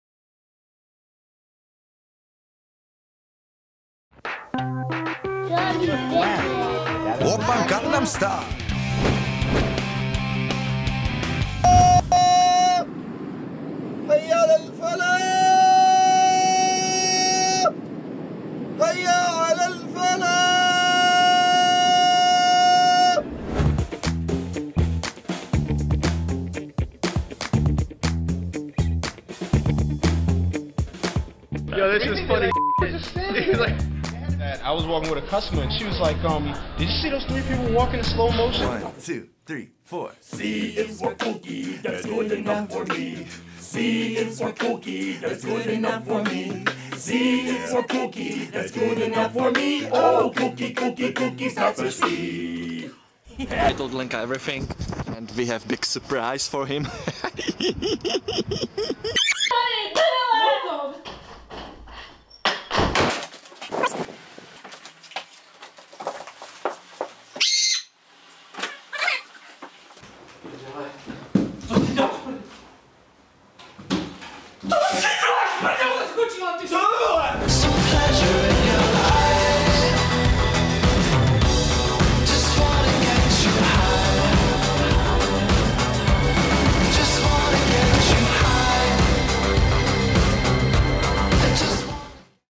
Trending Now 06/19/2014
The newest OKGo music video wraps up today's list of viral videos on Trending Now!